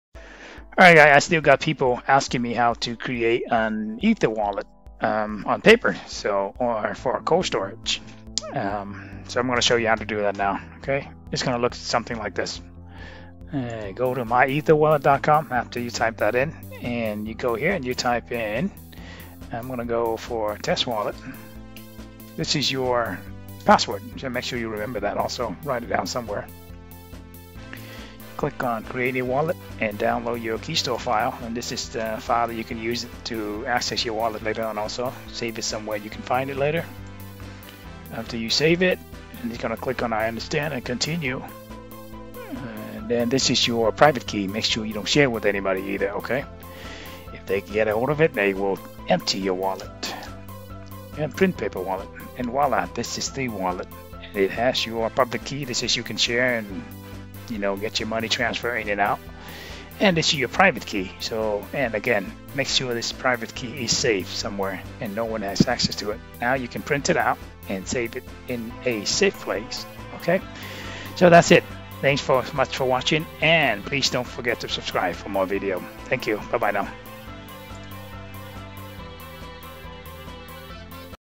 FREE How to Create an Ethereum Paper Wallet (cold Storage)
How to Create an Ethereum Paper Wallet (ERC-20 standard) in less than 30sec FREE.  This is as safe as Trezor, Ledger, Keepkey, etc... using MyEtherWallet.com AKA ICO wallet.

or you can click here to set up an Ethereum, Bitcoin, Bitcoin Cash, Litecoin wallet with Coinbase  and get $10 FREE*